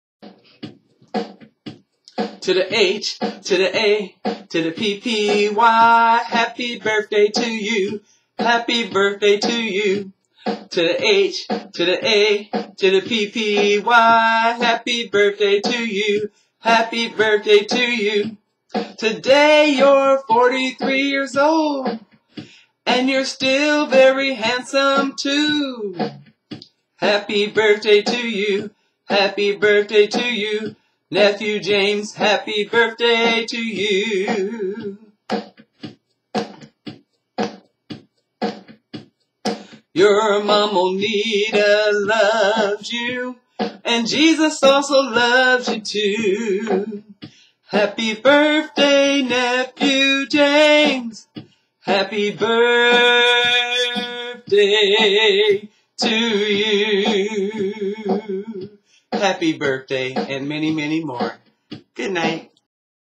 Happy 43rd birthday James
singing to my nephew James